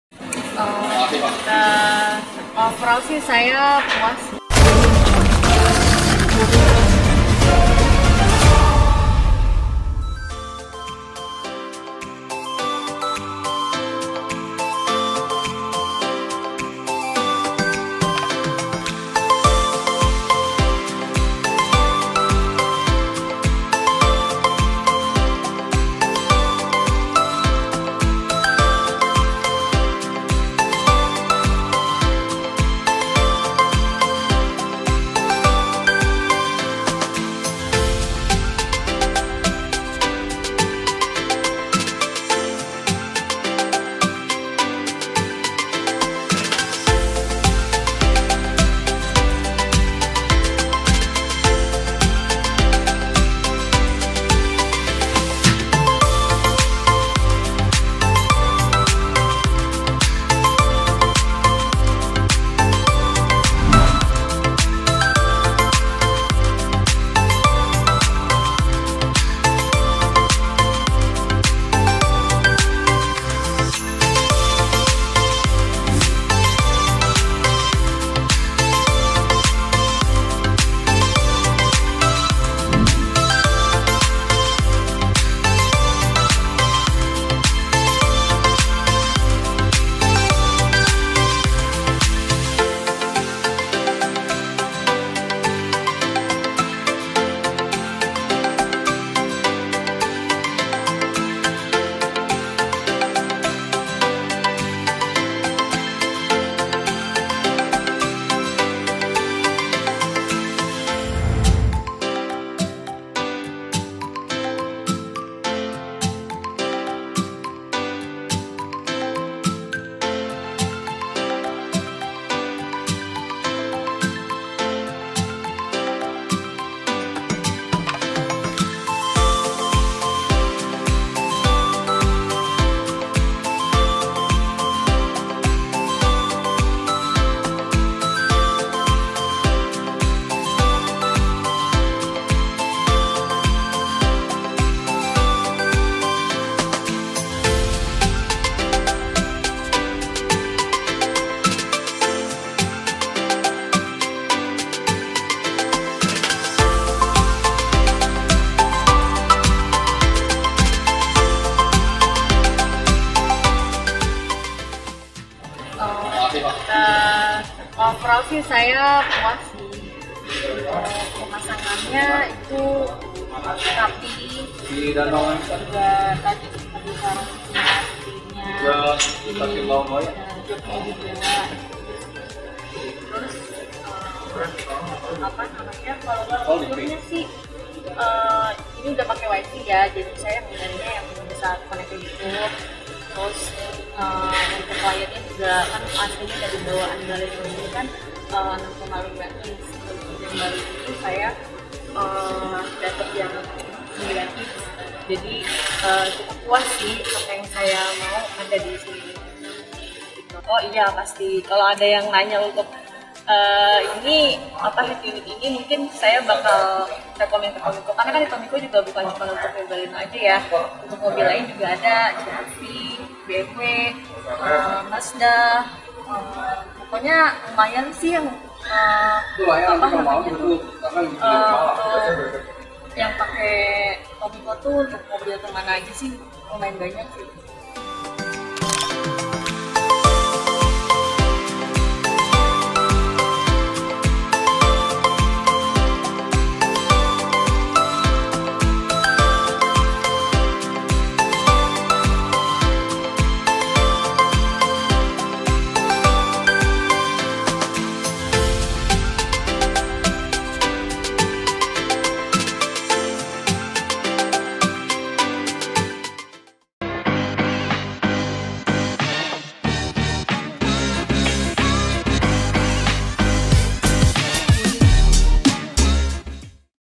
Pemasangan dan Testimoni Head Unit Android TOMIKO OEM for BALENO
Hallo Tomiko Lovers... 🙋‍♂️

Semakin nyaman berkendara dengan head unit android Tomiko,
Dilengkapi dengan berbagai fitur unggulan yang dapat memenuhi kebutuhan anda saat berkendara 😀

Terima kasih kepada pemilik mobil Baleno Ka Fila yang telah mempercayai Tomiko sebagai sarana hiburan dimobil anda😎

Spesifikasi:
OS : Android 8.0 Oreo
RAM : DDR3 4GB
ROM : 64GB
Support Hardisk 1 TB
9" IPS Screen
Screen : Capacitive and multi-touch
Support Steering Wheel Control
TV Streaming
Built in DSP
Mirrorlink Support Smartphone & iphone
Support Car Play
Support SIM Card Dongle (Data Connection)

Dapatkan di toko-toko audio terdekat di kota anda😎

Tetep semangat dan selalu jaga kesehatan... 👌